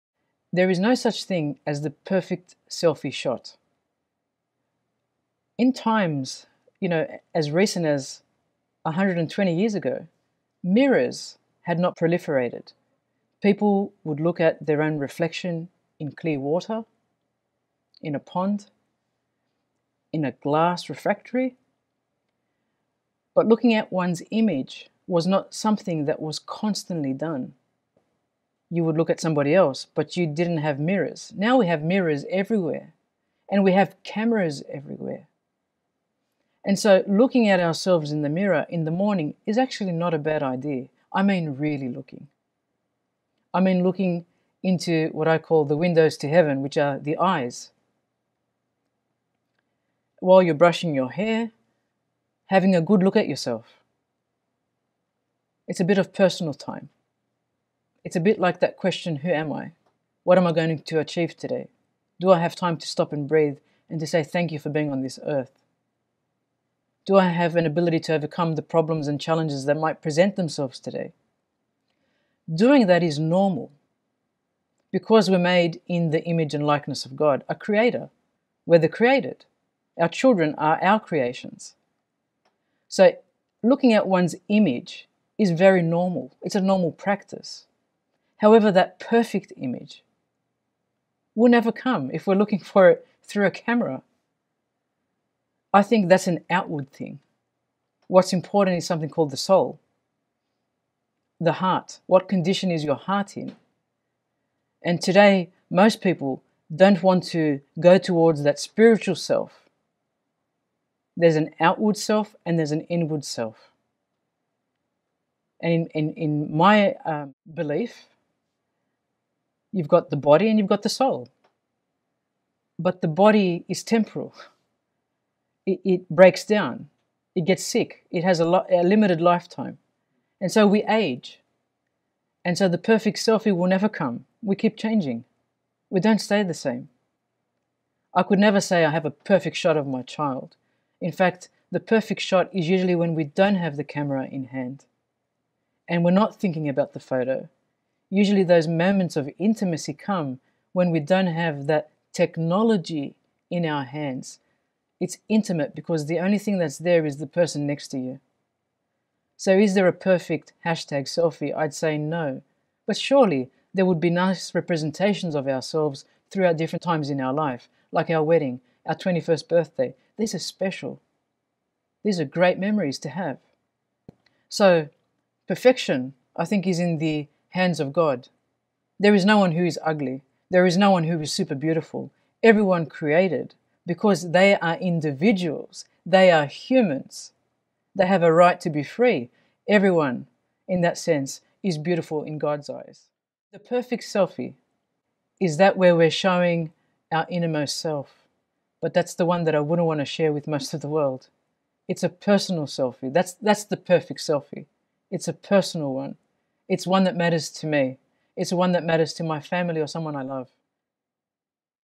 Digital Photographs, the Perfect Selfie and Being Made in the Image and Likeness
A series of videos filmed on 21 July 2015 at the University of Wollongong when I was the Associate Dean - International in the Faculty of Engineering. The short clips explore Social Media Addiction from a variety of perspectives. The videos were used for a pilot considered for ABC TV. Recorded and filmed by the dedicated staff at Conti Bros Films in Australia.